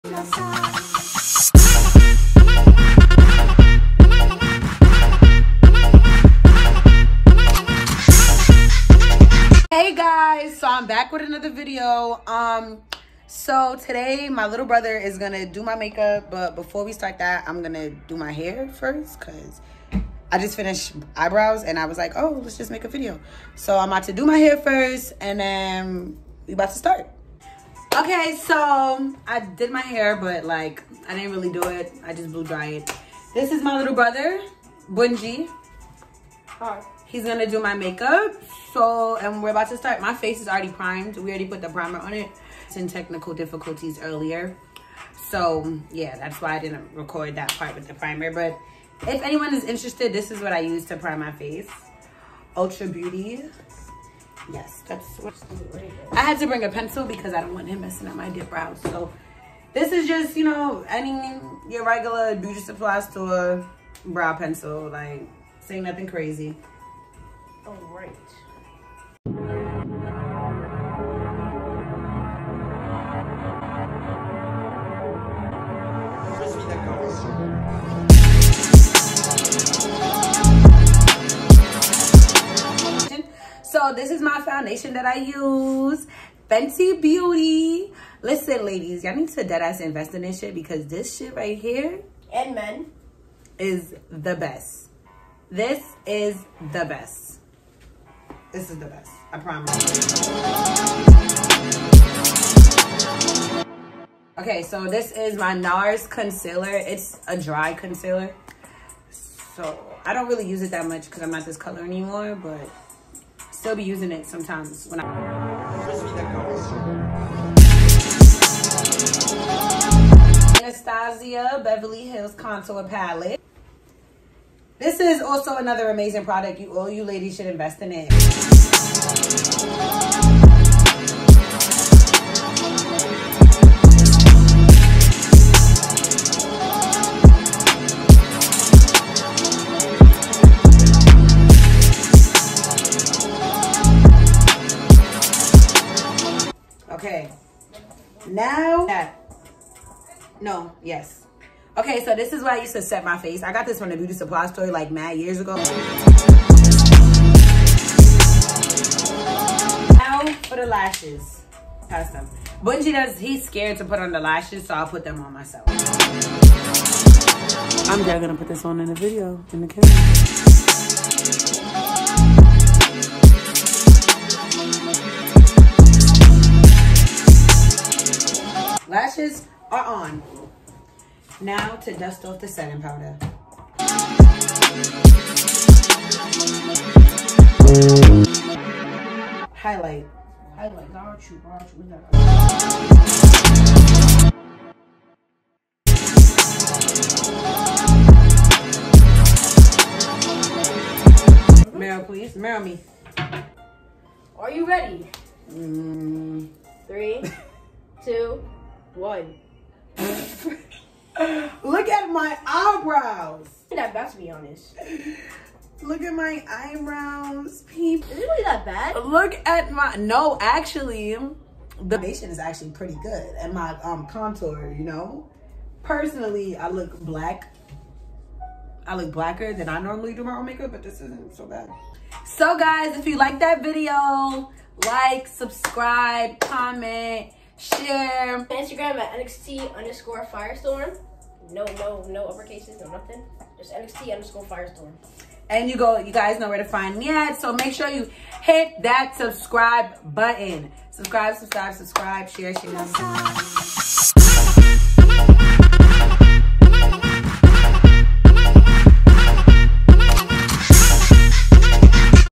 Bunji Beats my Face : Little Bro Does My Makeup
Watch My Baby Brother Either Fleek 🤩 Or Grease 🤬 My Face

Products Used :
1. Eyebrow Pencil (Color Jet Black)
2. LA Pro Girl concealer (Cool Tan/Fawn)
3. Fenty Beauty Pro’Filtr Foundation (420)
4. Anastasia Cream Contour Palette (Medium to Dark)
5. MAC Shiny Pretty Things Face Compact (Medium to Dark )
LASHES : ENVY BEAUTY SUPPLY STORE BRAND